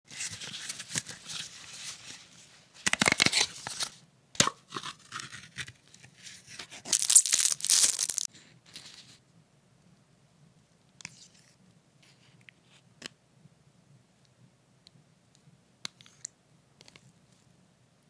【短縮版】 森永 とろふわプリン 濃いバニラ
森永乳業の「とろふわプリン 濃いバニラ」です。イオンにて購入。